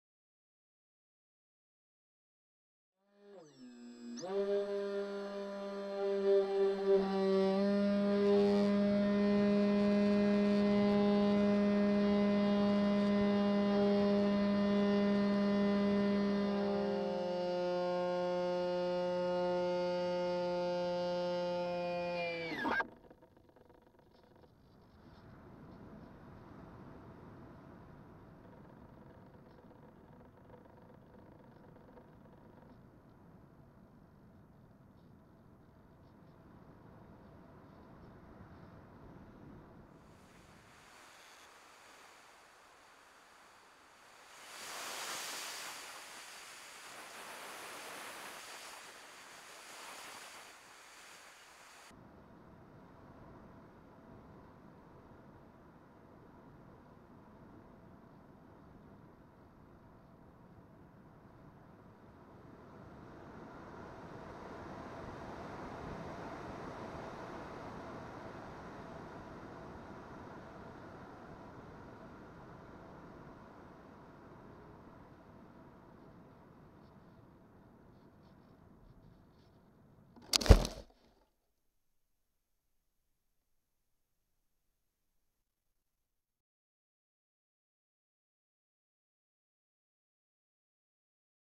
Death of the SunnySky
After only 15 flights, the SunnySky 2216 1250 kv got too hot and had a meltdown